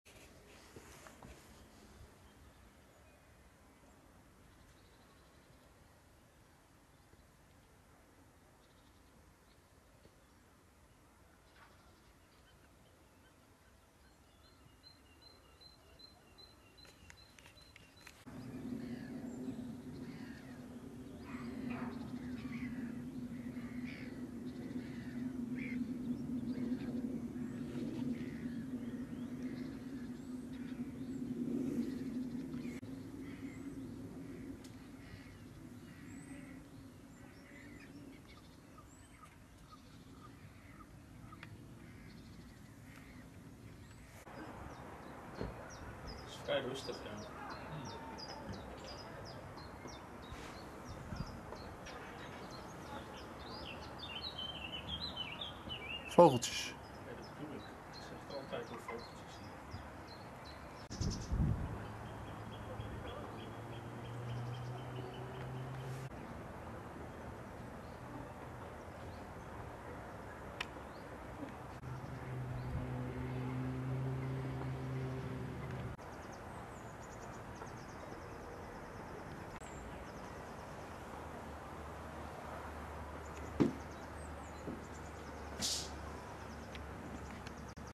Megahit Natuur in Stad Grunn.
Eerst volkstuinencomplex Golden Raand. Daarna ergens in Zuid. In April 2o19.